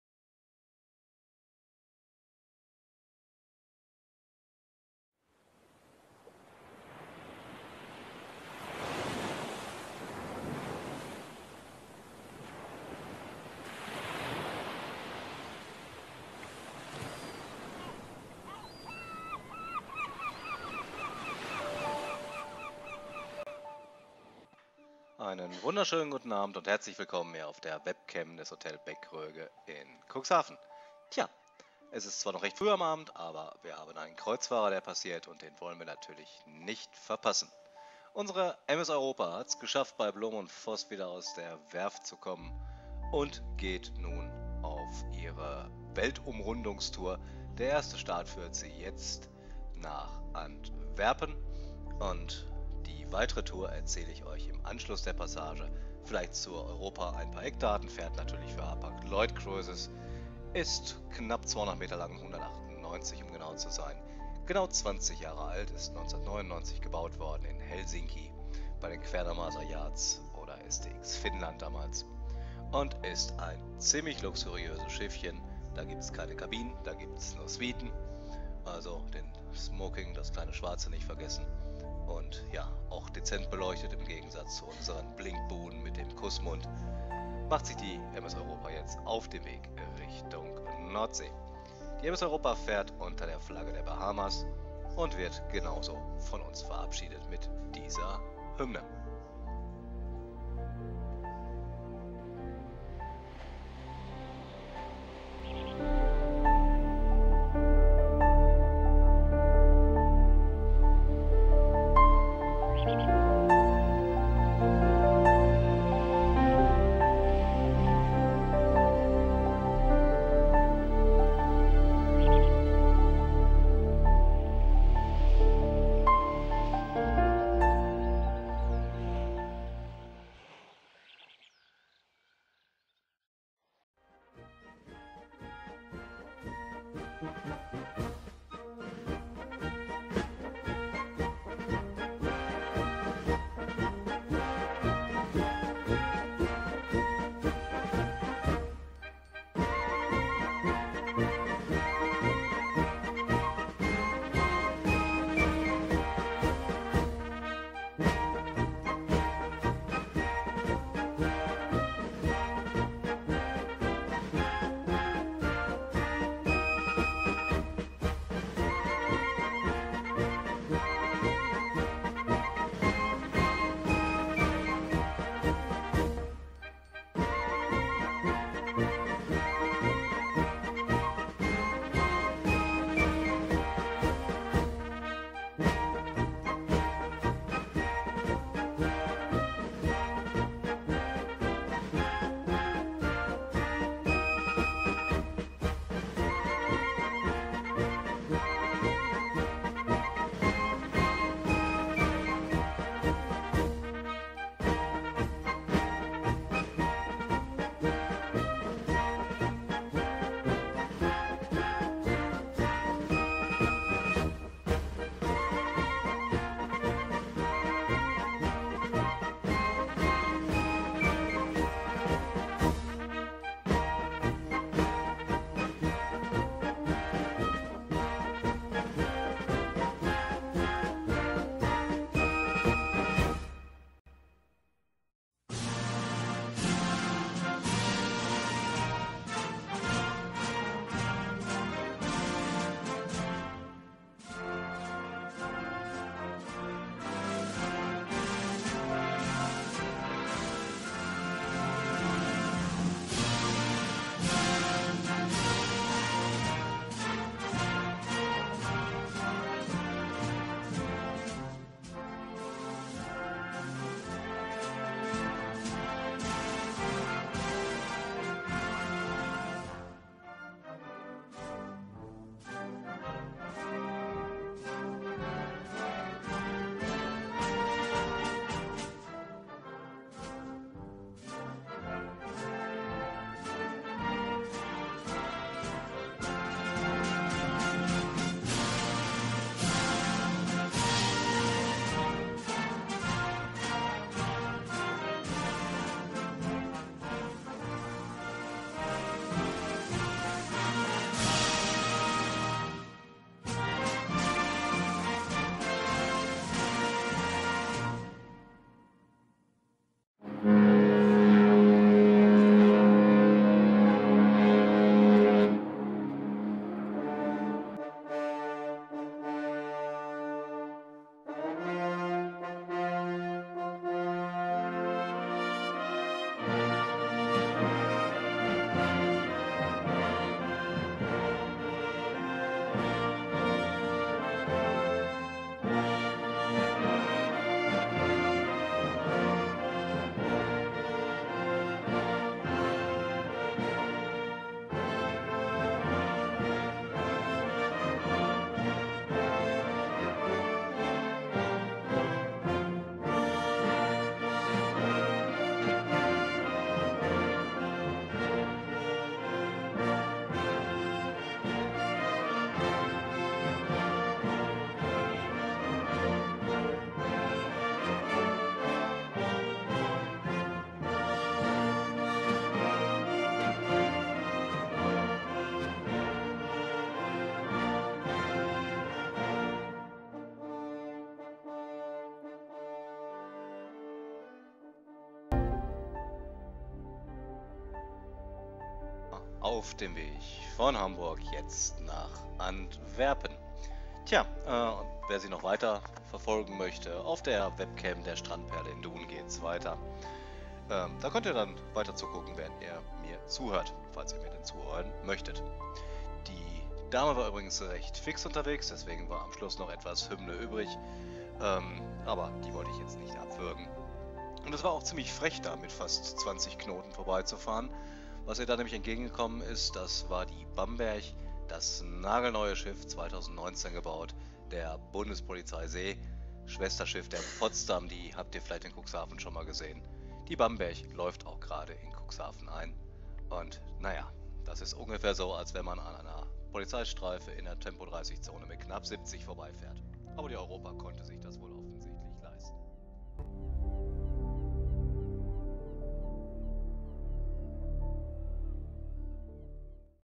MS EUROPA geht auf Weltreise
Die EUROPA, das 5 Sterne + Schiff von Hapag Llloyd, kommt aus der Frischzellenkur bei Blohm + Voss in Hamburg und möchte nach Antwerpen.